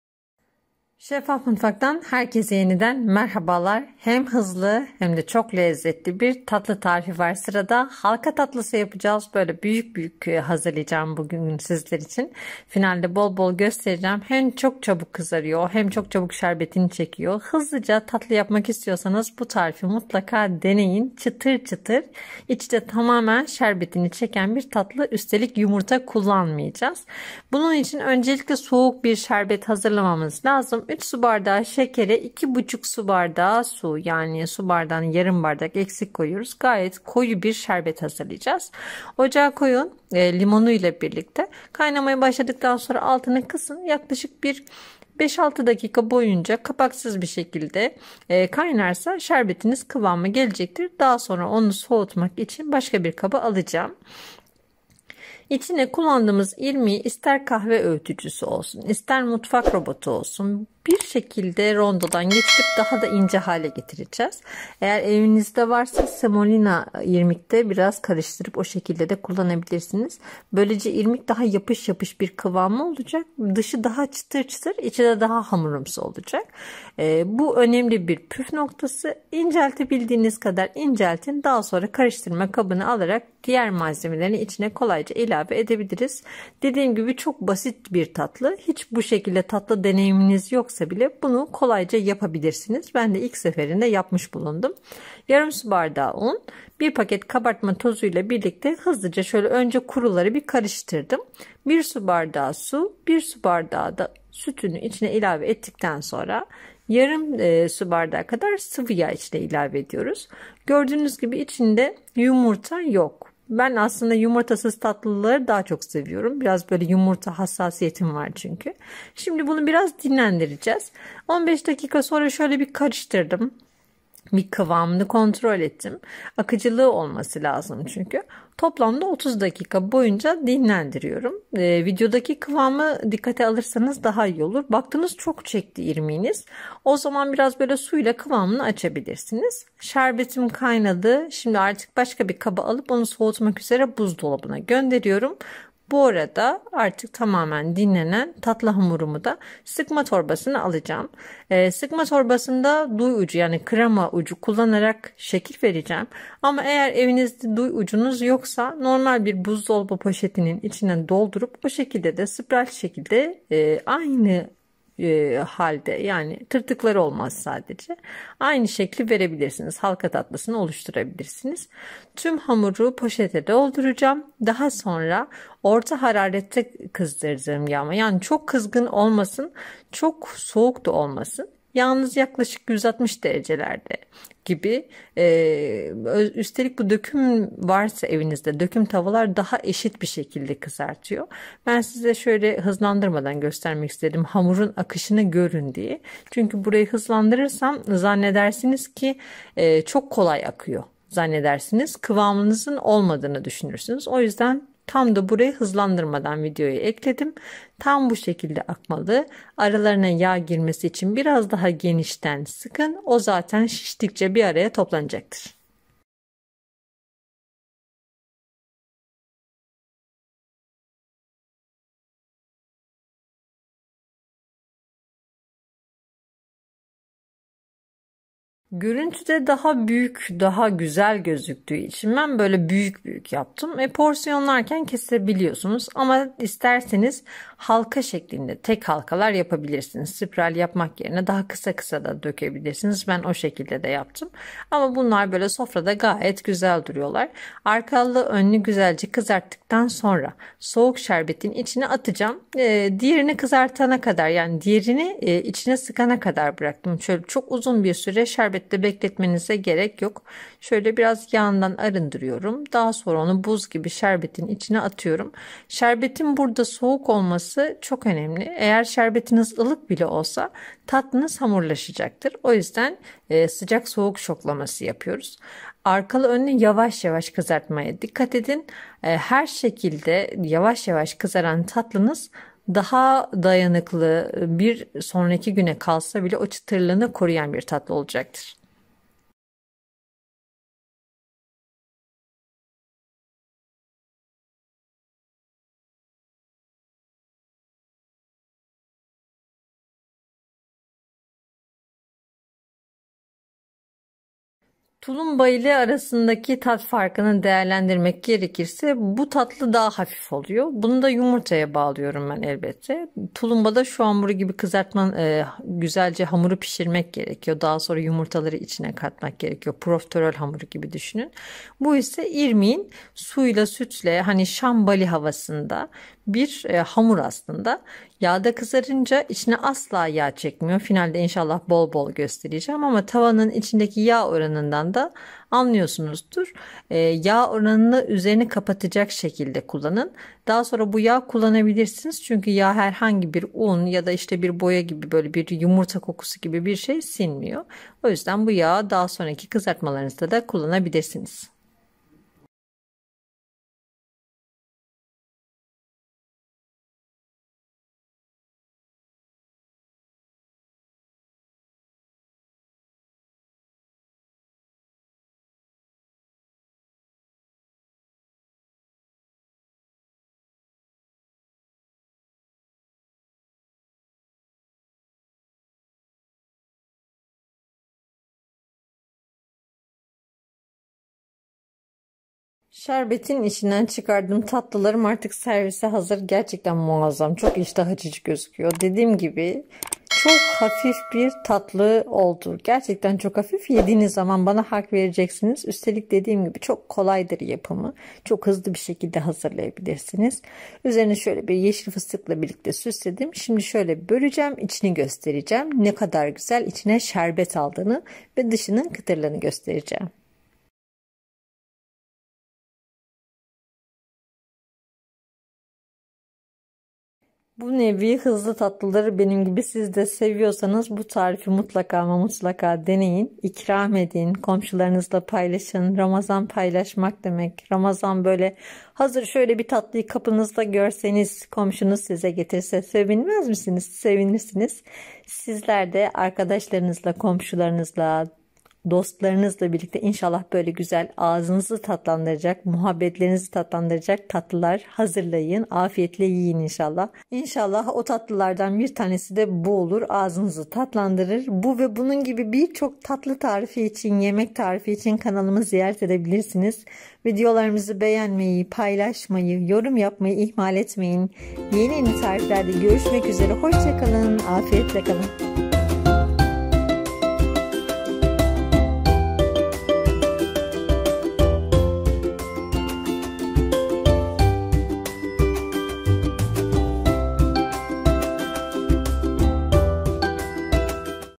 Mutlaka Sende Dene 👉🏻Az Malzemeli Garantili Halka Tatlısı 💯 Üstelik Çok Kolay 👍🏻
Herkesin yapacağı kadar kolay üstelik çok da hızlı bir tatlı 💯

İçinde yumurta yok bu yüzden çok hafif 🤗🤗

HALKA TATLISI

şurup
3 su bardağı şeker
2,5 su bardağı su
çeyrek limon

500 gr irmik
Yarım su bardağı un
1 su bardağı süt
1 su bardağı su
2 yemek kaşığı şeker
1 paket kabartma tozu

Kizartmak için sıvı yağ